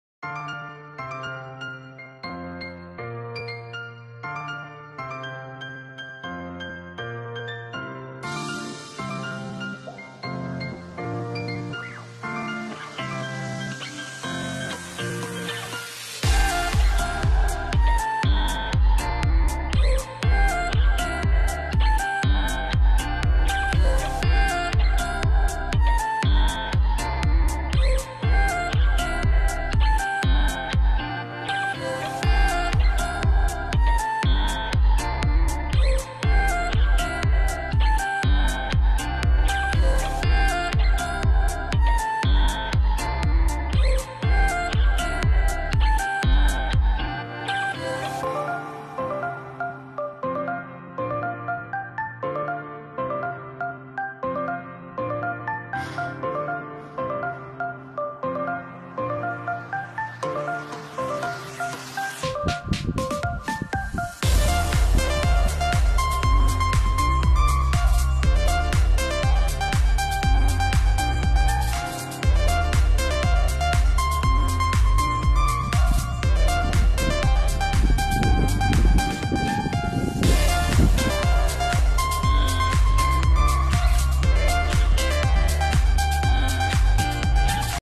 Video1 D.I.Y RECYCLED LAMPSHADE Elegant and Minimalist Design
Old water containers to a very elegant lampshade!